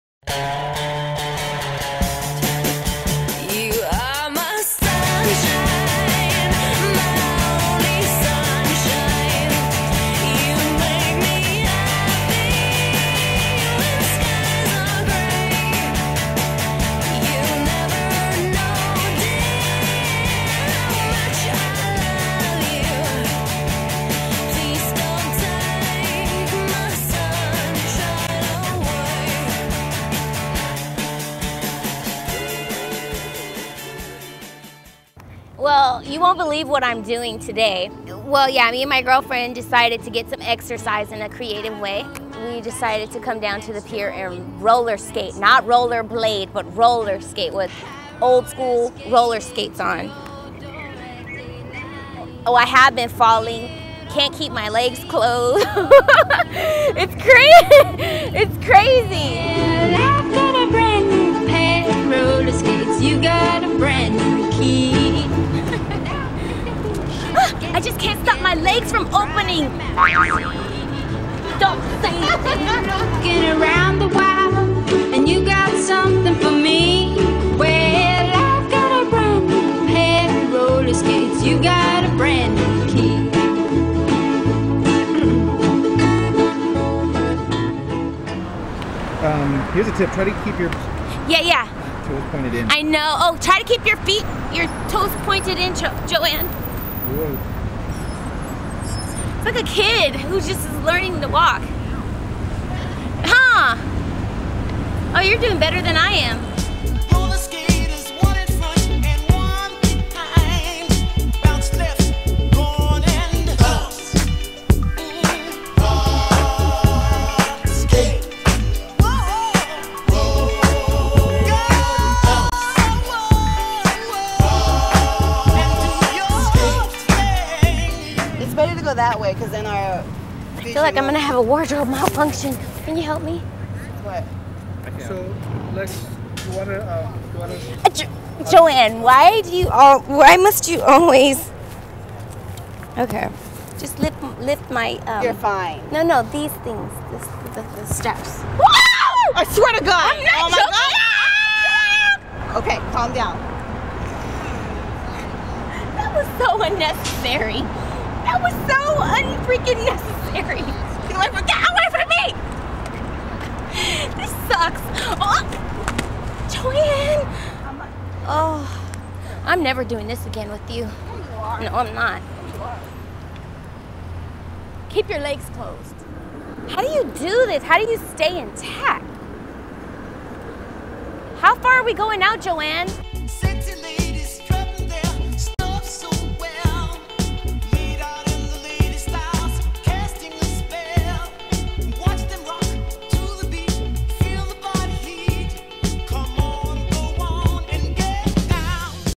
Rollerskating, Pt. 1
Joann and Tina strap on their skates and take on the terrain at SF's Embarcadero!

Watch in High Quality for best results.